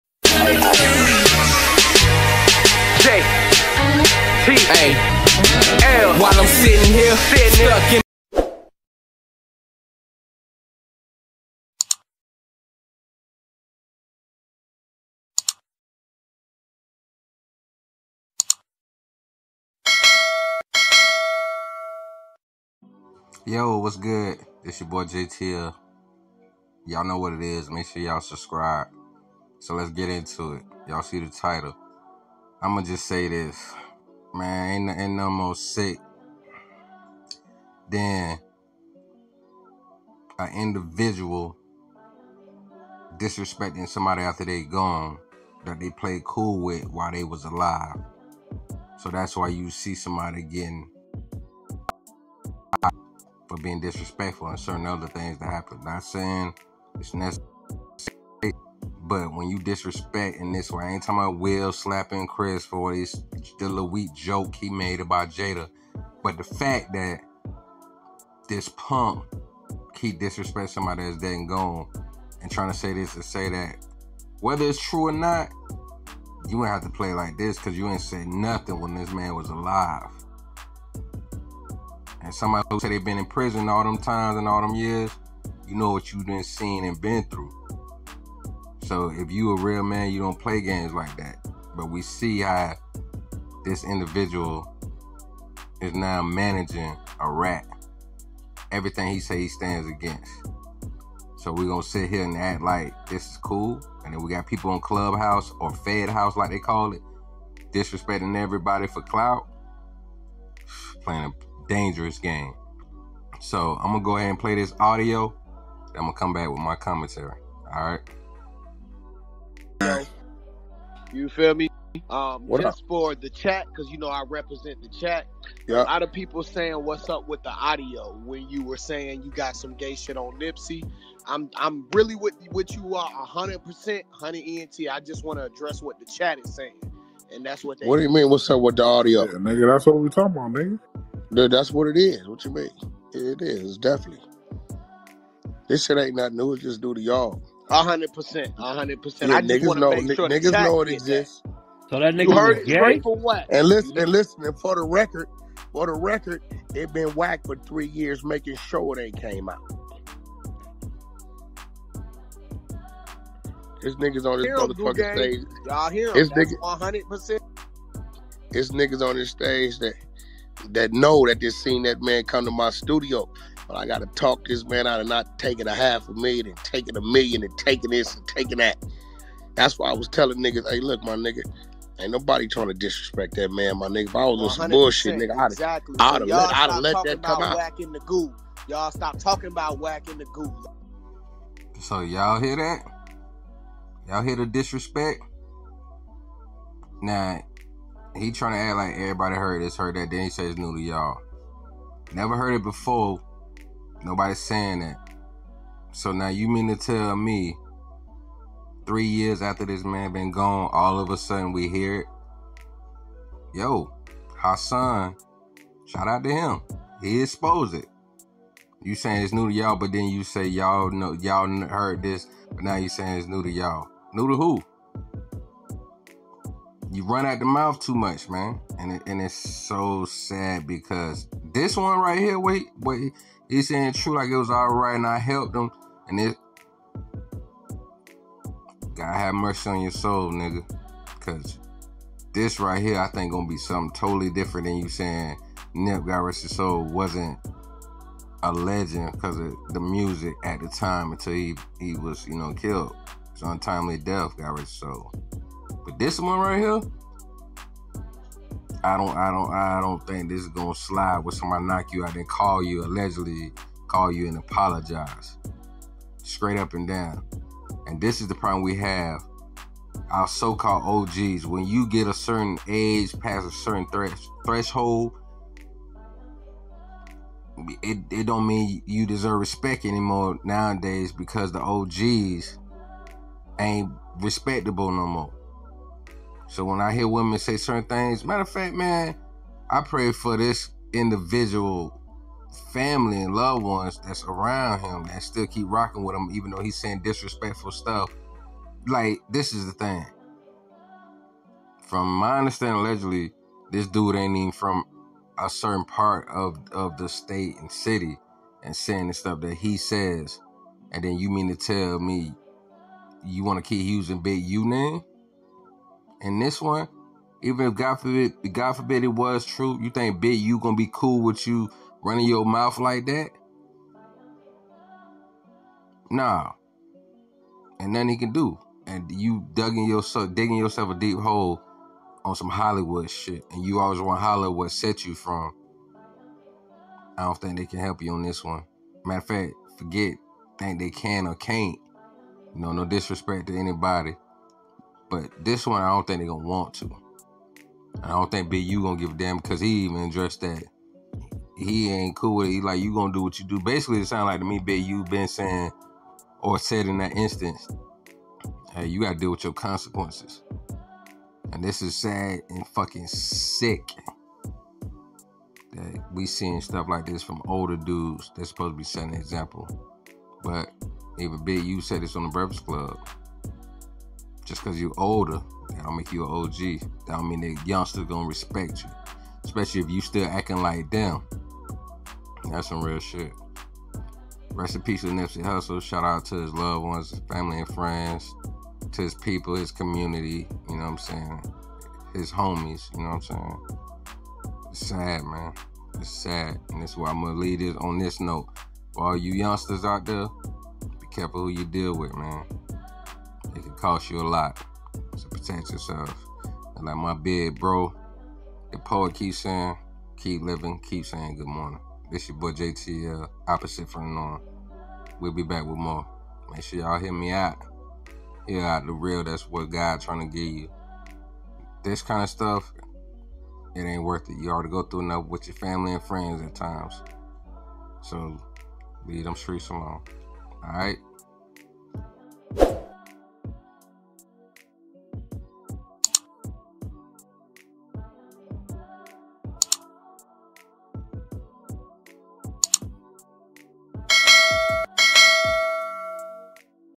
wack100 says he kept Nipsey Hussle from being exposed as g#y? smh
sad wen a supposed og acts like a lil boy and don't forget when you listen to this video that nip baby mama is big U niece, so wack disrespectful to a few people right now .